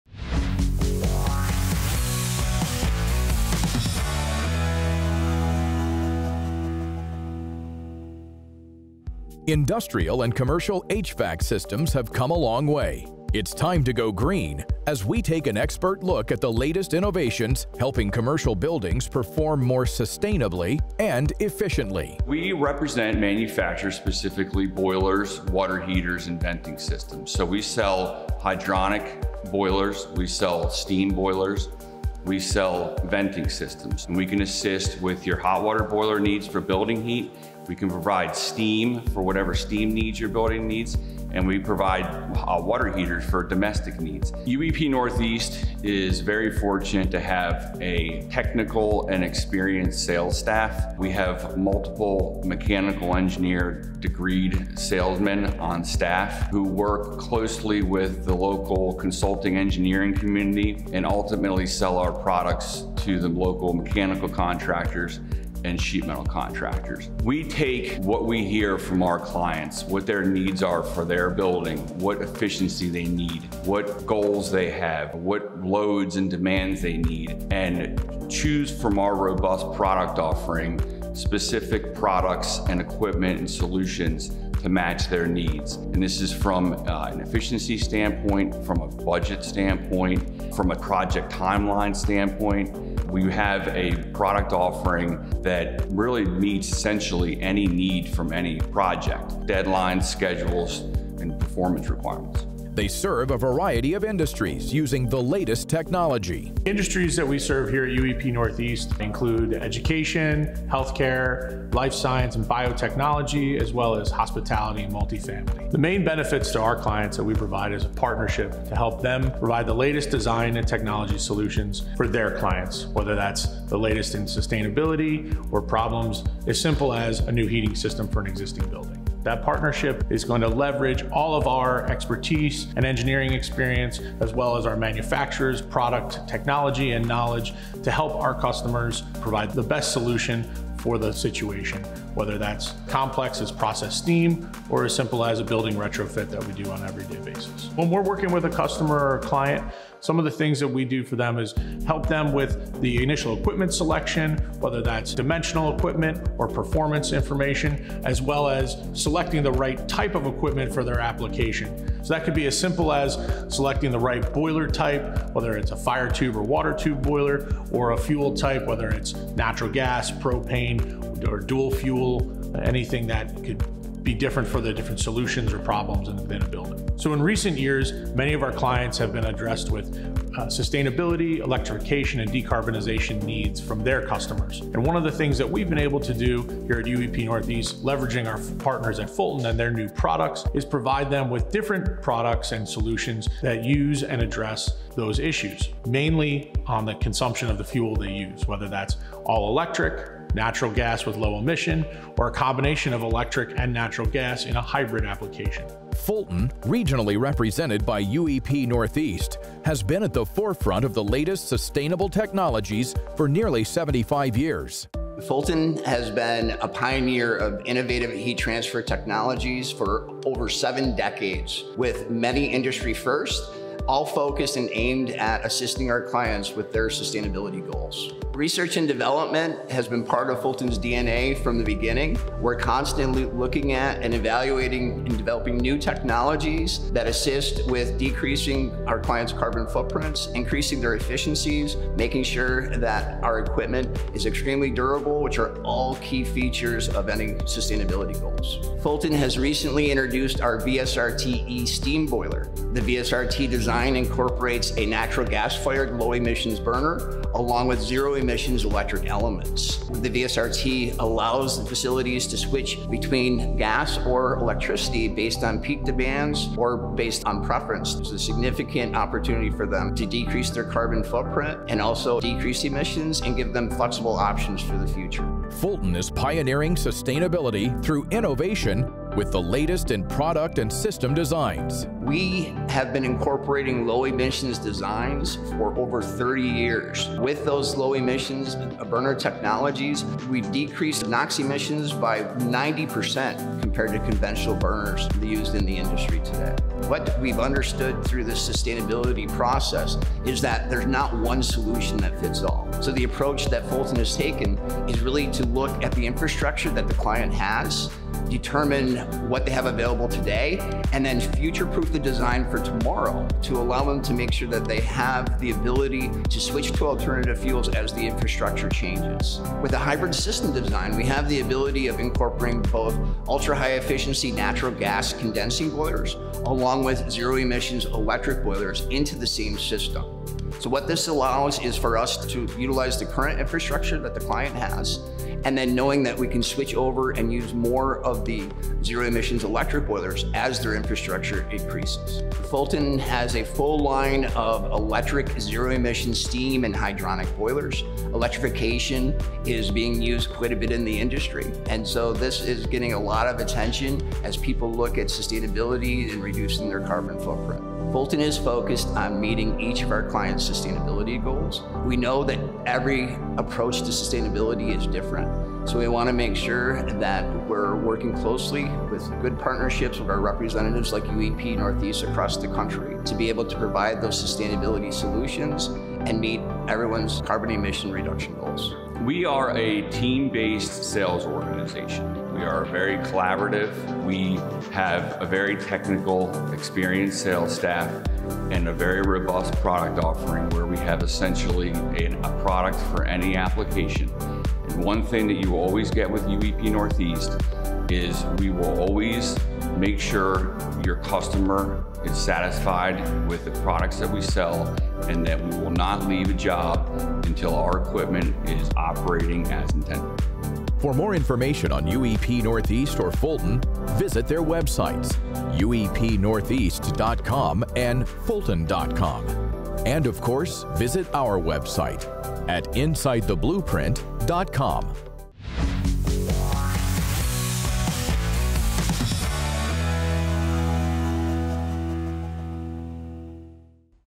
An Expert Look At Innovations in Industrial & Commercial HVAC
Industrial and commercial hydronic and steam systems have come a long way. It’s time to go green as we take an expert look at the latest electrification and decarbonization innovations helping businesses work more efficiently and meet their sustainability goals with the help of UEP Northeast. Fulton, regionally represented by UEP Northeast, has been at the forefront of the latest sustainable technologies for nearly 75 years.

BSSTOR BSLC 0564